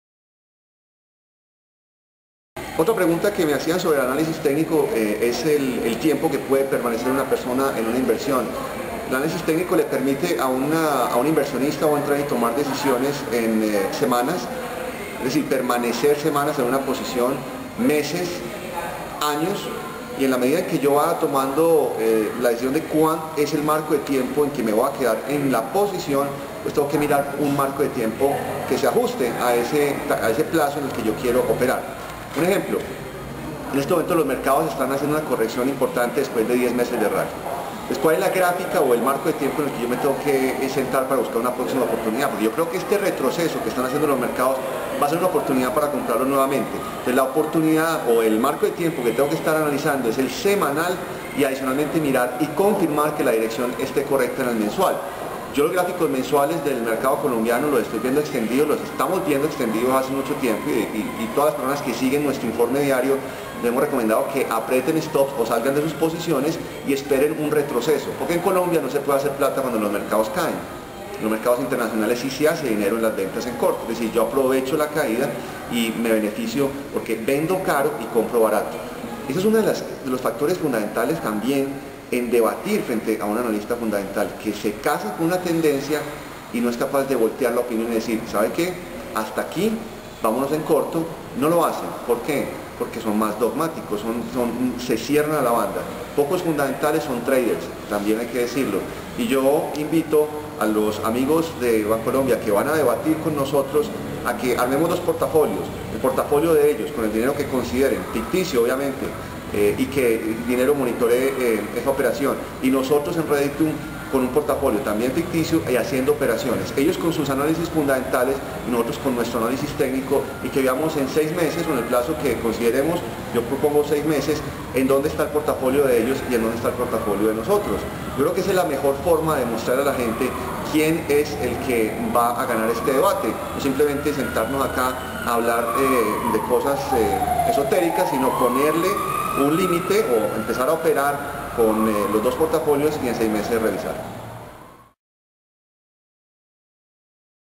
Propuesta Juan Villegas, análisis técnico vs. fundamental
Juan F. Villegas, Gerente de Reditum, propone un ejercicio en donde se pueda comparar el Análisis Técnico con el Análisis Fundamental.